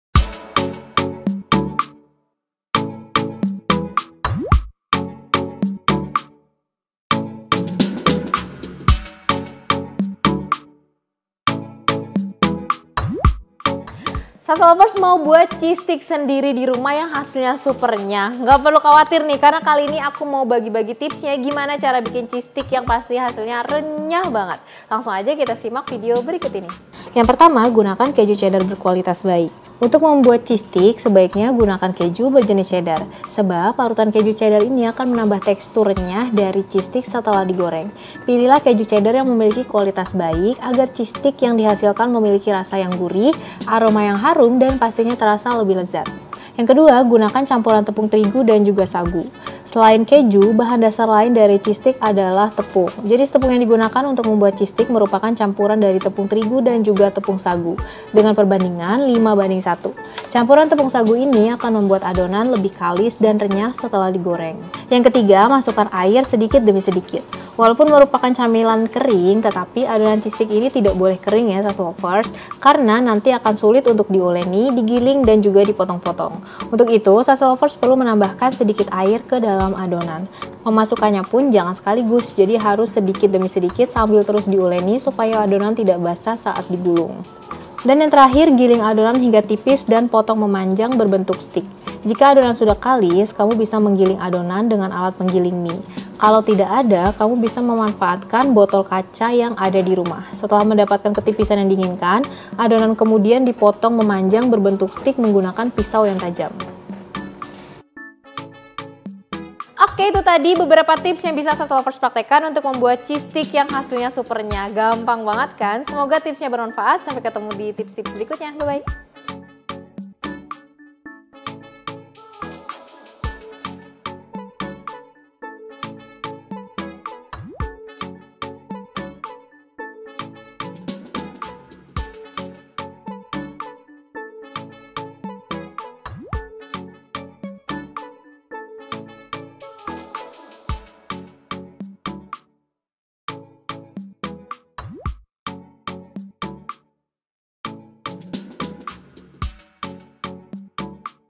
TIPS MEMBUAT CHEESE STICK YANG RENYAH ALA BAKERY TERKENAL
CARA MEMBUAT CHEESE STICK YANG RENYAH ALA BAKERY TERKENAL, TERNYATA SEMUDAH INI!

Bahan:
175 gr tepung terigu protein sedang
30 gr tepung beras
15 gr tepung maizena
1/2 sdt garam
1 sdt baking power
150 gr keju cheddar parut
25 gr margarin
2 btr telur
500 ml minyak goreng

Produser Eksekutif: Intan Yusan Septiani
Penulis Resep: Resi
Penulis Naskah: Resi
Peraga: Resi
Fotografer: Osyad
Videografer: Osyad
Video Editor: Osyad